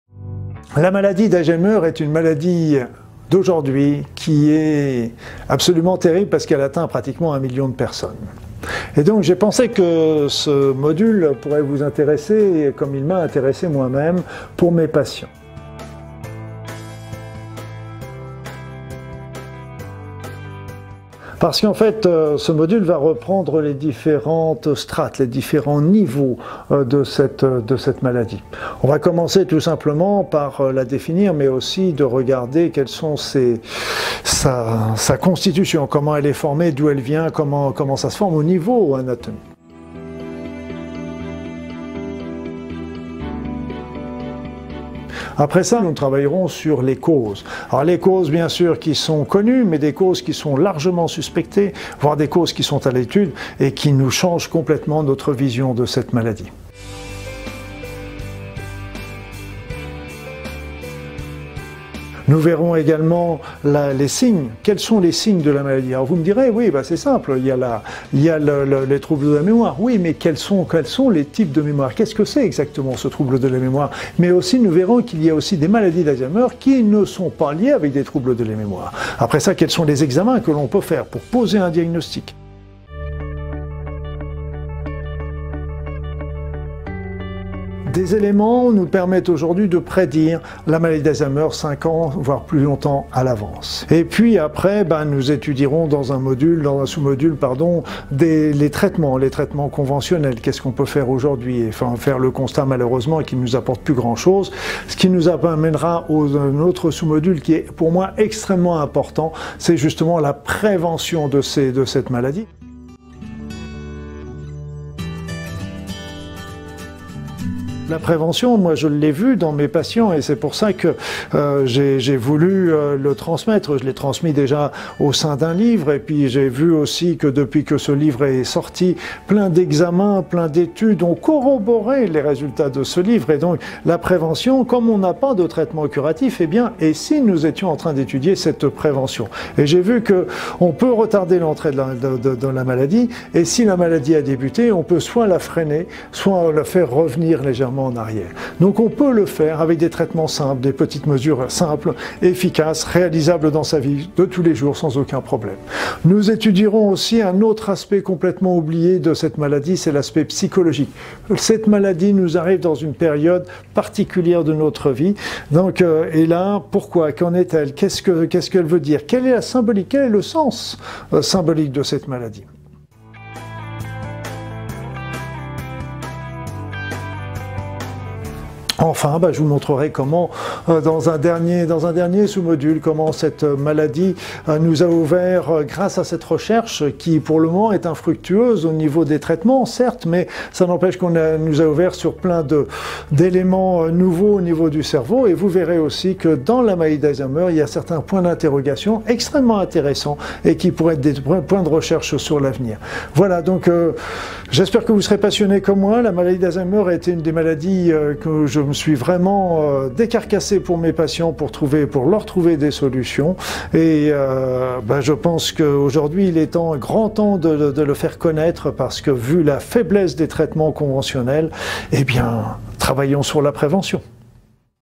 Prévention de la maladie d'Alzheimer par Luc Bodin
Signes & Prévention de la Maladie d'Alzheimer par Luc Bodin.

Luc Bodin est un ancien médecin. Il a exercé la médecine pendant 25 ans. Il est donc parfaitement conscient des questions que chacun se pose sur la maladie d'Alzheimer : "Mes trous de mémoires ne sont-ils pas les signes d'une maladie d'Alzheimer ?" Mais aussi : "J'ai perdu les clés de la maison... j'ai oublié le nom de mon voisin... est-ce un début d'Alzheimer ?" Etc. C'est pour répondre aux inquiétudes de ses patients que Luc Bodin s'est passionné pour la maladie d'Alzheimer.